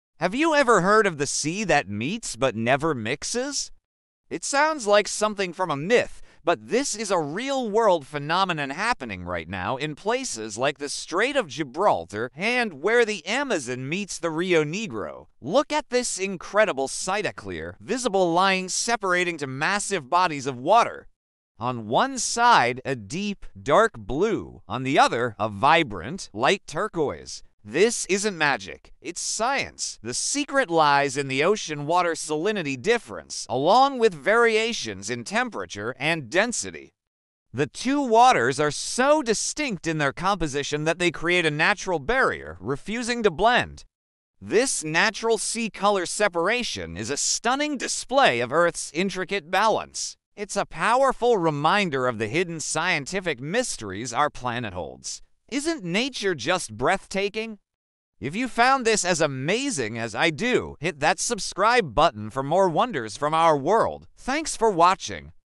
Two Seas That Never Mix… The Mystery Nature Can’t Explain 🌊✨
Have you ever seen two oceans meet but never mix? 😳🌊
In this video, discover one of the most unbelievable natural wonders on Earth — where two seas come together yet remain perfectly separate.

Why doesn't the water mix? 🤔
Scientists point to differences in salinity, density, and temperature…
But nature still keeps its secrets 👀✨

If you enjoy mysterious natural phenomena, hidden places on Earth, and mind-blowing facts — this video is for you ✅

👇 Don’t forget:
📌 Share with someone who loves nature mysteries!